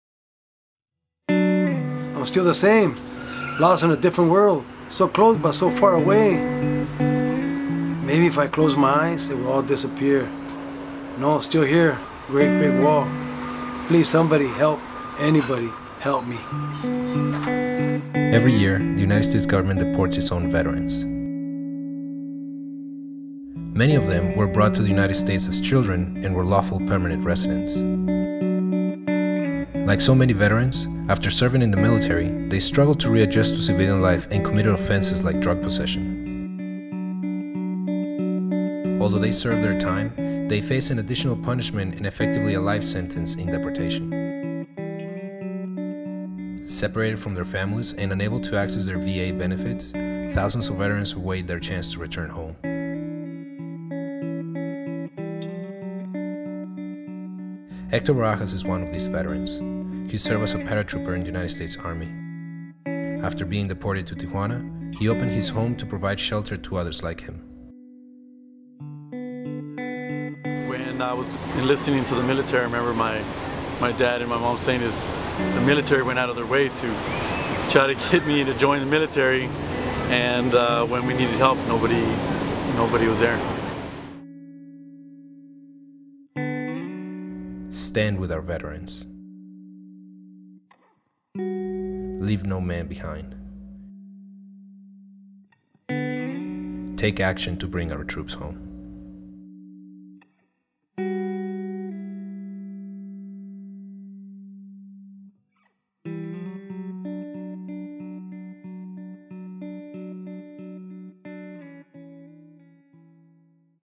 Bring Them Home
If you're willing to sacrifice your life for this country, you should be able to call it home. Take action and tell Congress to bring back our deported veterans